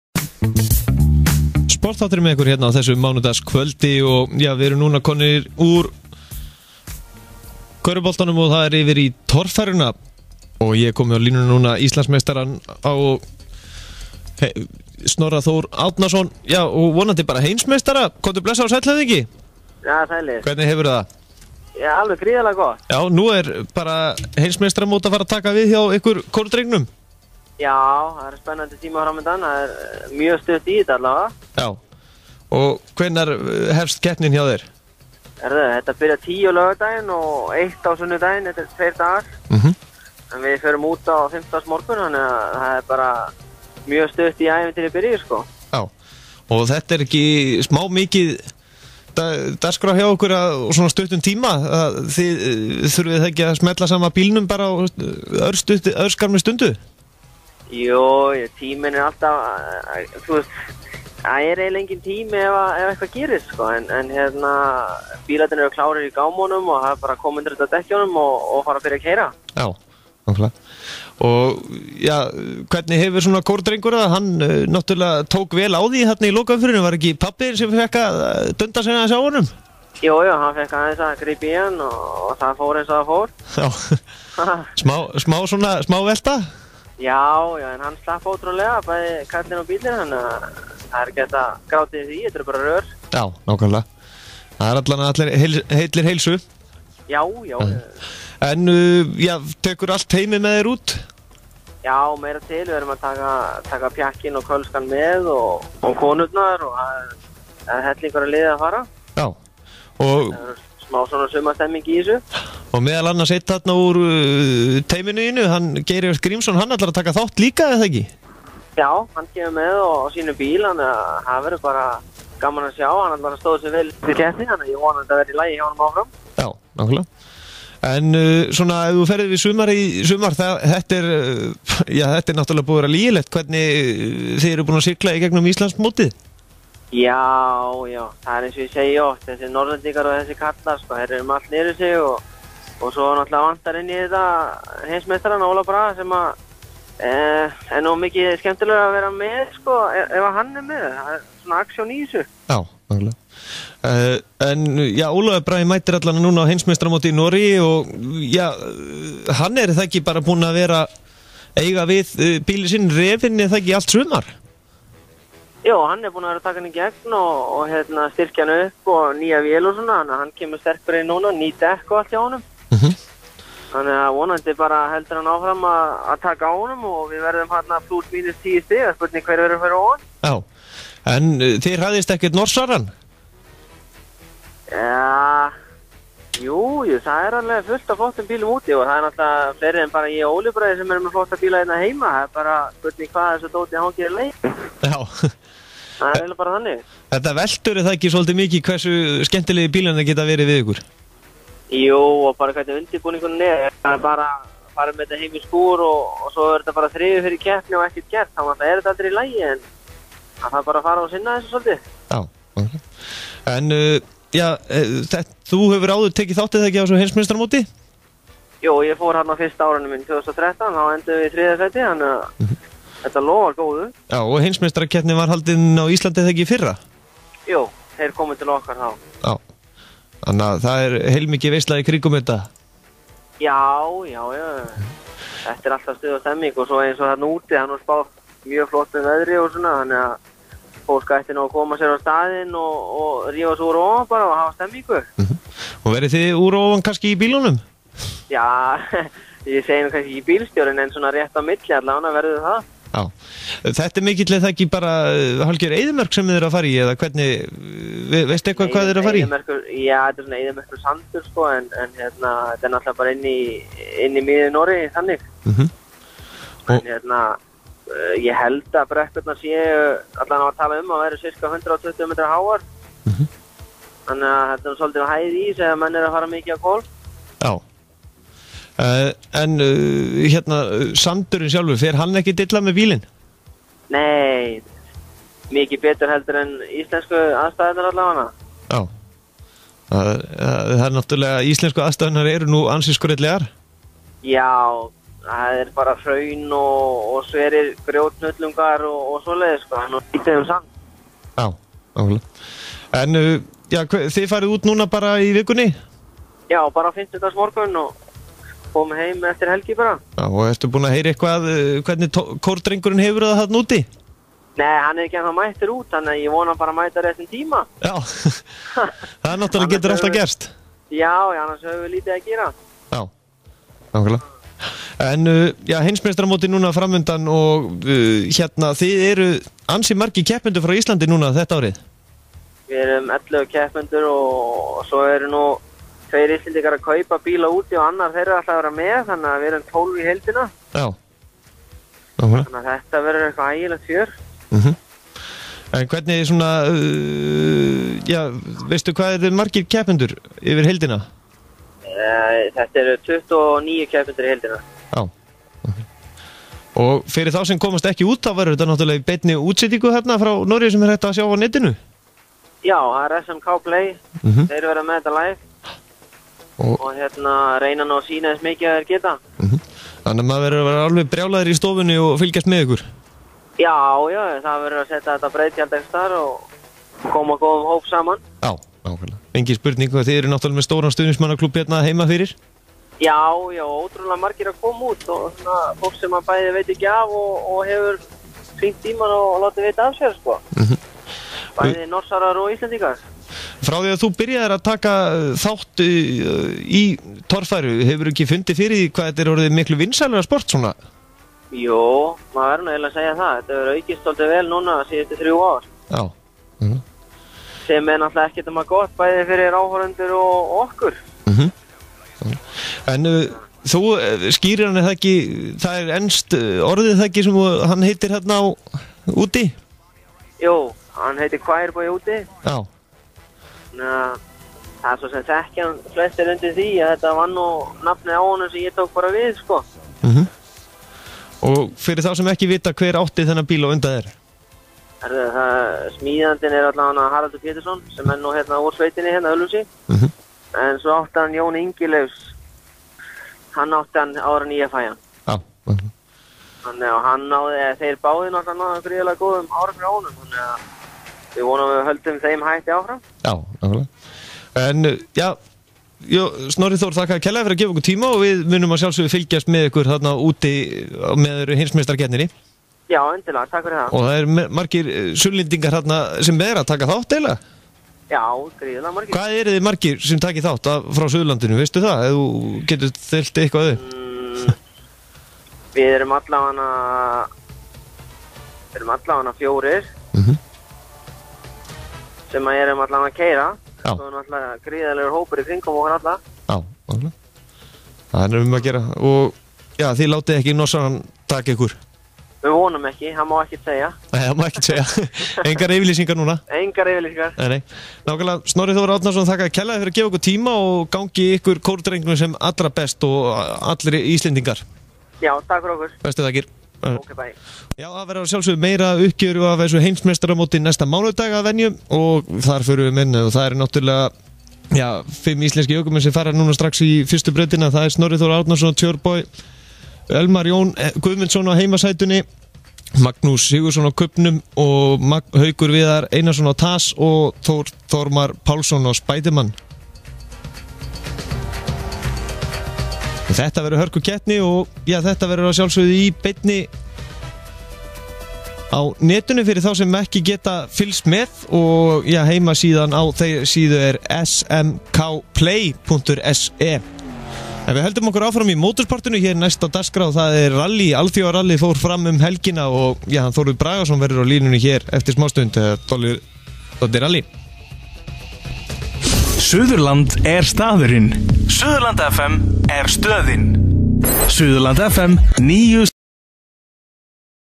Sportþátturinn - Snorri Þór Kórdrengurinn Íslandsmeistari
Sportþátturinn Mánudagskvöld er stýrður af Gesti frá Hæli og er í loftinu öll mánudagskvöld frá kl. 20-23 á Suðurland FM. Nánar á facebook "Sportþátturinn Mánudagskvöld"